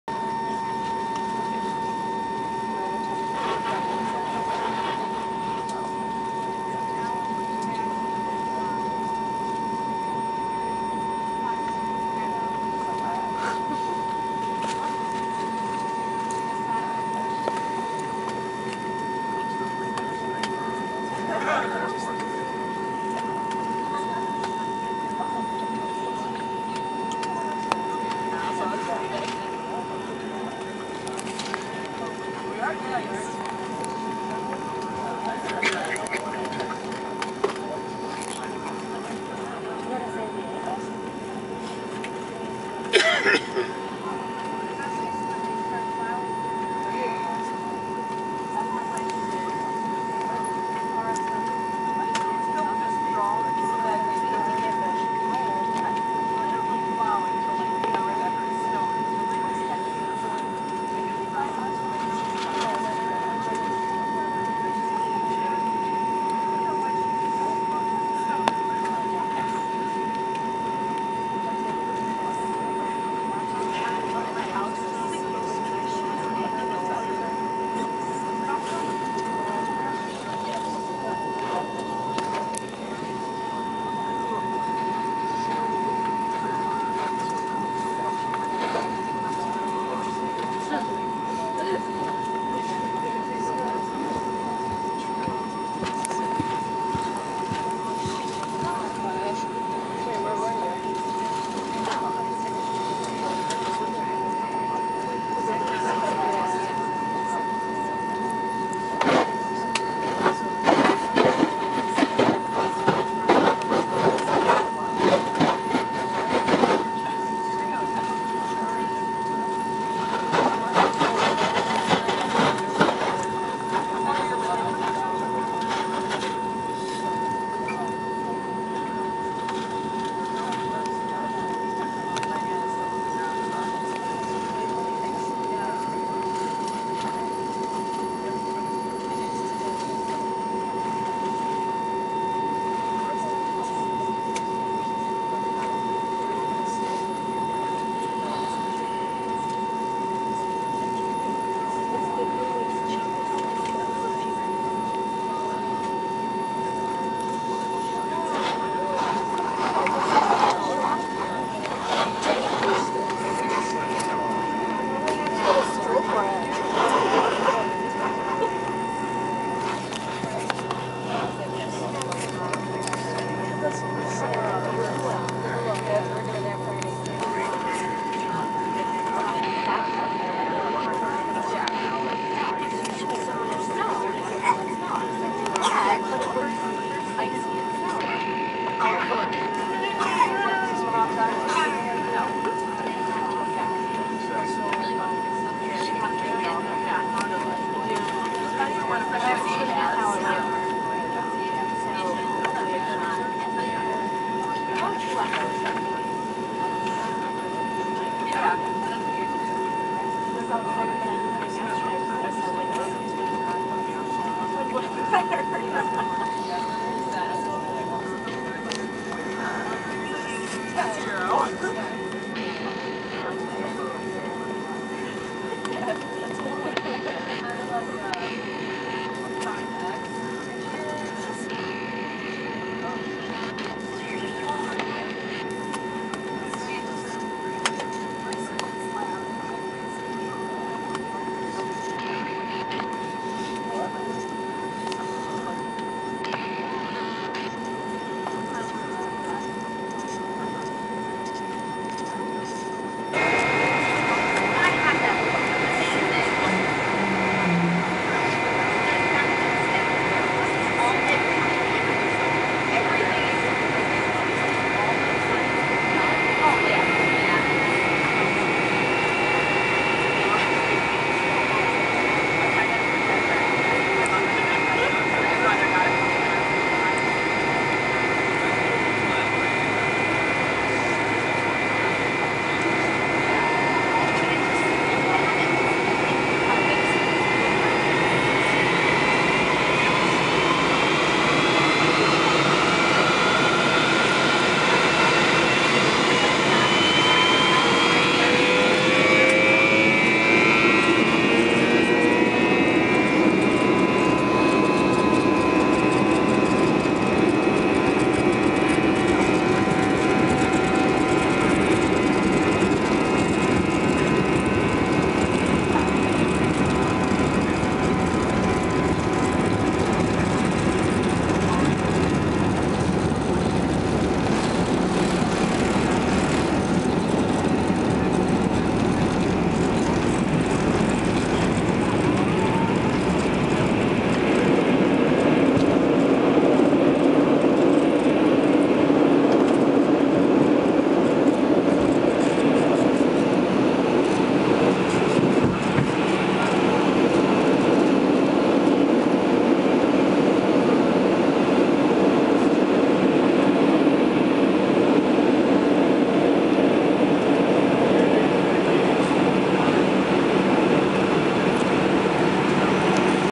Delta MD-88 De-icing and Takeoff at Rochester Int'l Airport
Due to editing errors, I had to re-upload the video. Enjoy a view of Rochester's climate. After an hour waiting on the tarmac for the runway to be plowed, we finally got de-iced...
The temperature there was 65 degrees 5 days after that video was filmed!